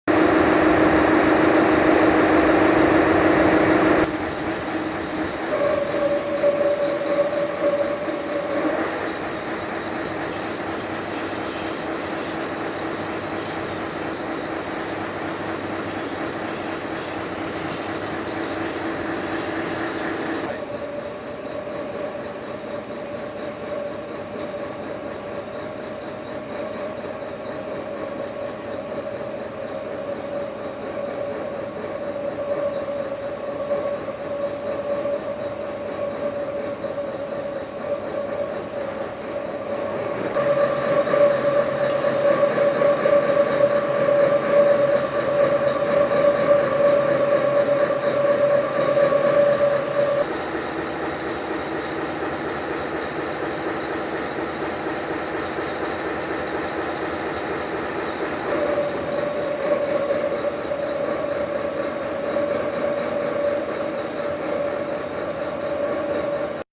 Get close-up to Ever Forward, still stuck in Chesapeake Bay
WBAL-TV 11 News gets an up-close look at the Ever Forward, the 1,095-foot container ship that grounded in the Chesapeake Bay near Craighill Channel on March 13.